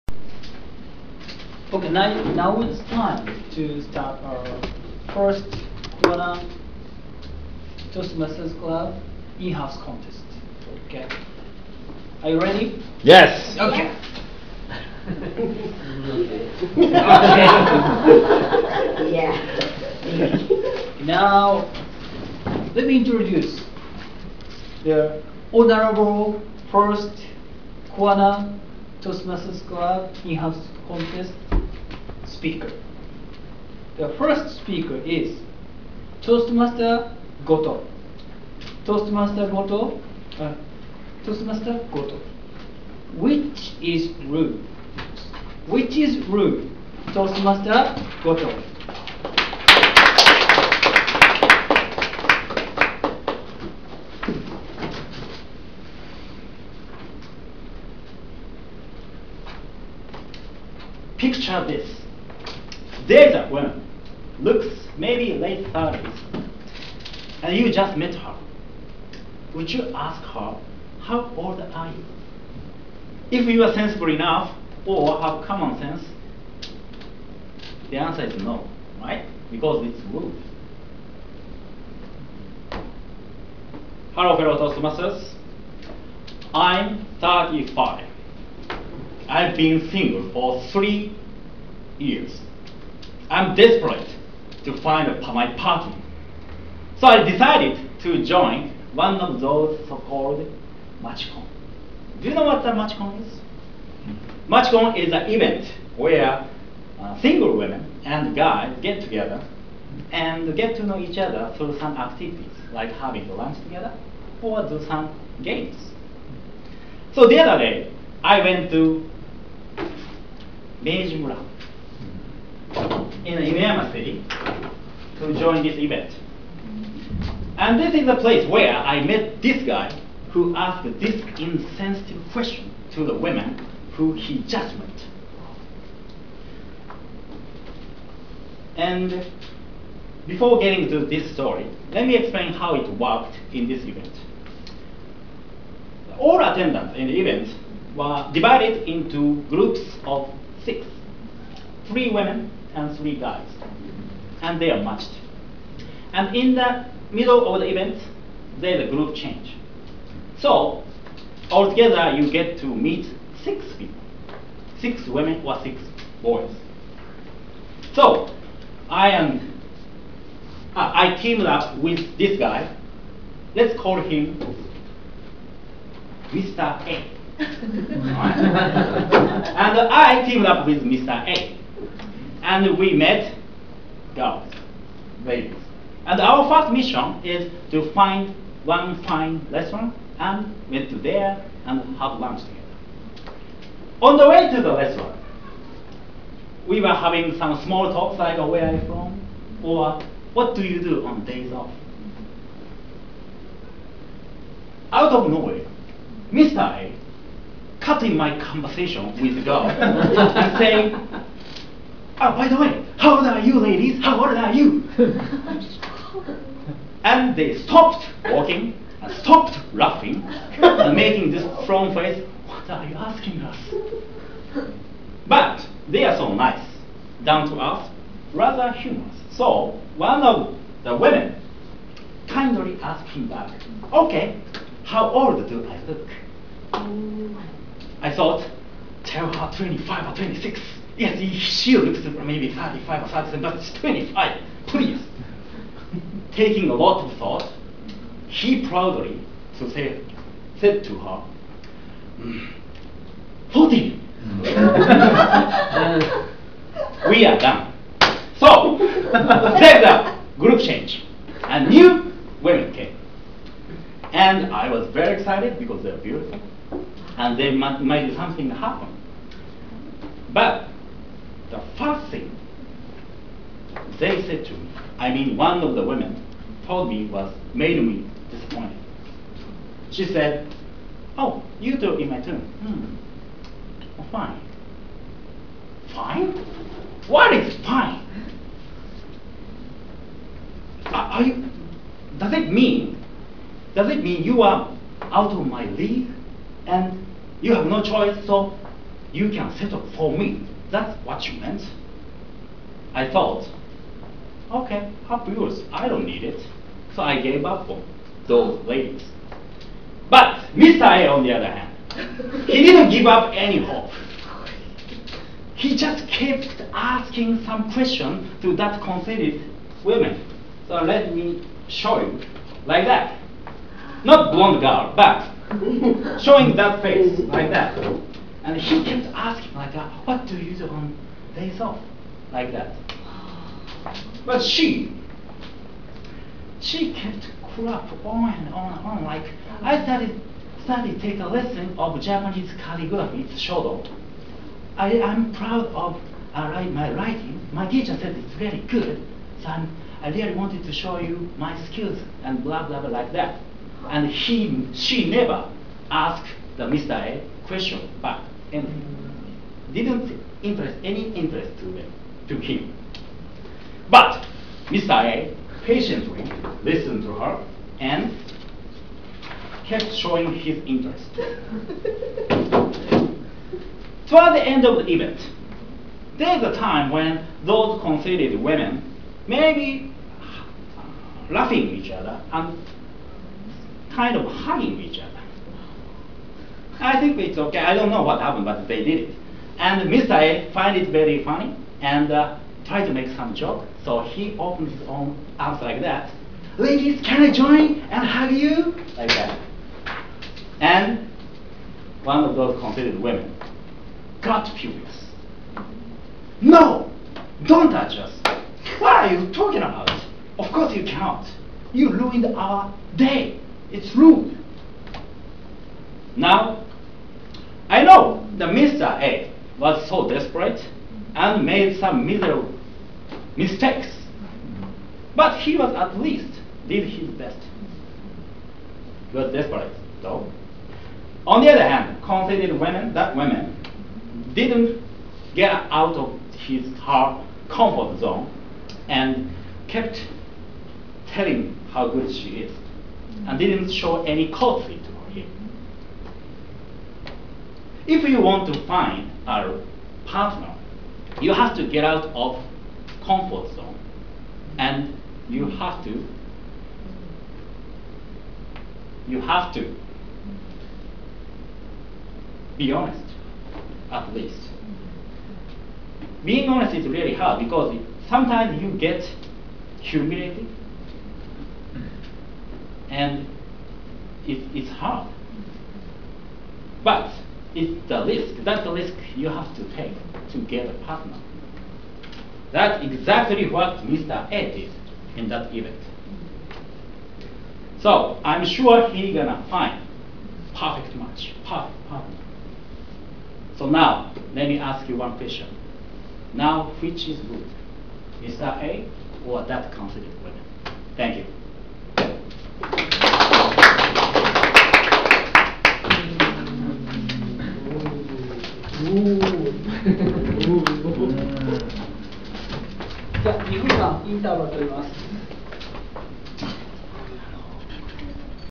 TM Goto Speech on Kuwana Toastmaster's private contest
It's been about 1 and a half year since Kuwana Toastmaster has been established. Since the club has not been chartered yet, it is not allowed to hold official contest. So we held the speech contest by ourselves to let all members experience how it feels like.

Speaker: TM Goto
Title: Which is rude?